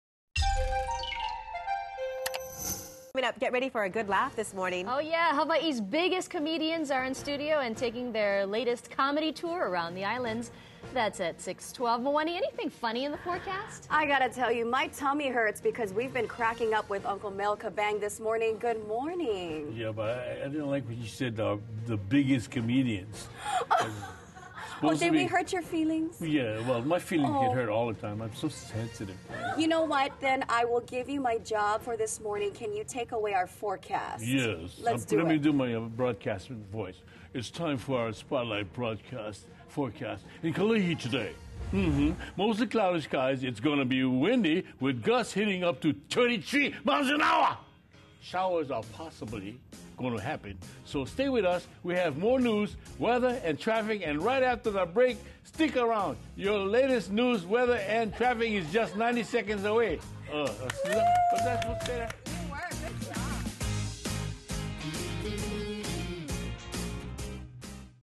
Spotlight Forecast: Kalihi
Expect mostly cloudy skies and windy. For news you need to know,